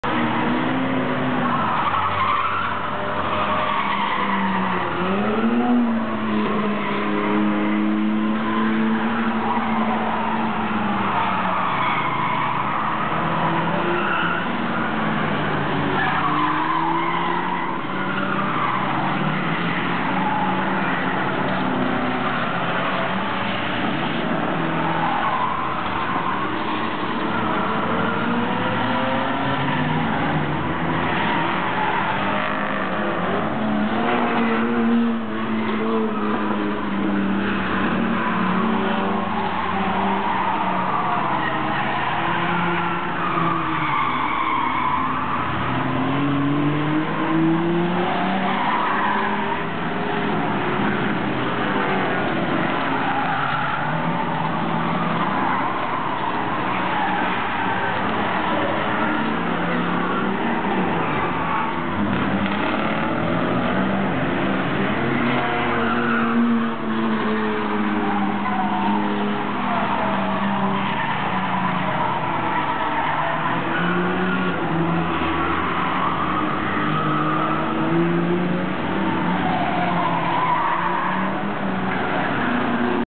24 hours of LeMons - Altamont Raceway - No. 11
Video footage from the 24 hours of LeMons race on Altemont Raceway, Oct. 7-8, 2006.

The crowd. The spectacle. The pall of blue smoke and roasted clutch discs. In all motorsport, no event captures the universal human need to whale on old crapcans and hoover down greasy barbecue like the 24 Hours of LeMons.

This endurance race is for cars purchased, fixed up, and track-prepped for a total of 500 bucks or less--and before reaching the grid, participants had to survive the Old-Lady Mannequin Slalom, the Baby-Carriage Braking Test, the Brick On the Gas Pedal Challenge, and similar qualifying rounds.

I mean, seriously--what's not to like?